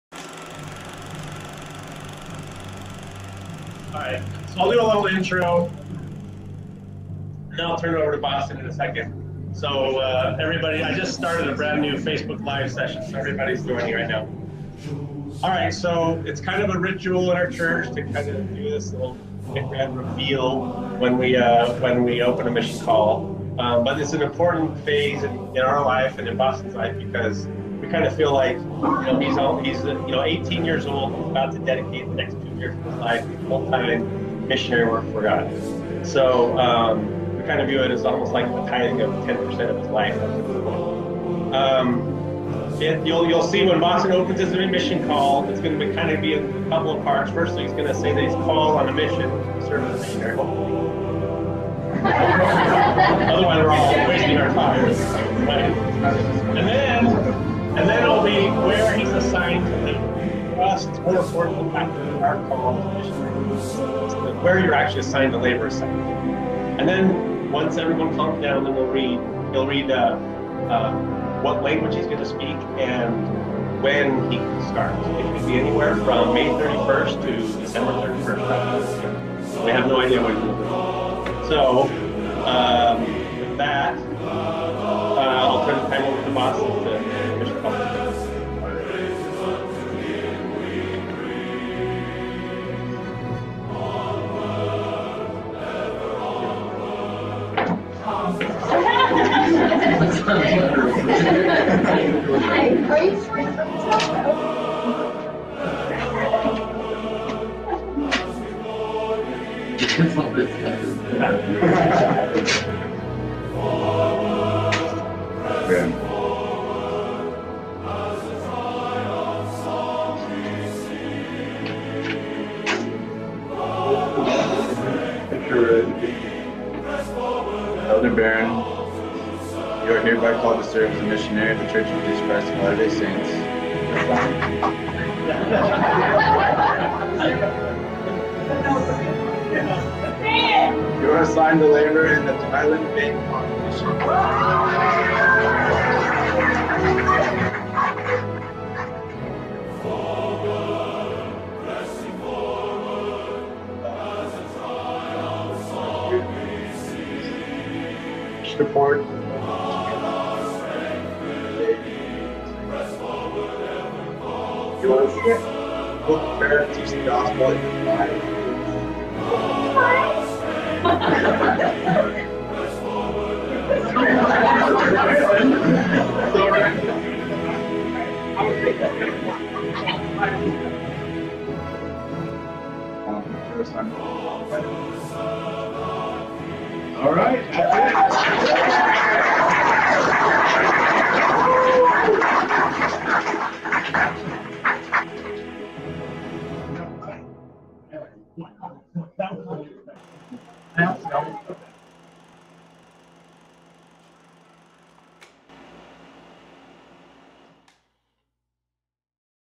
Boston opens his mission call
A very special day for Boston and our whole family.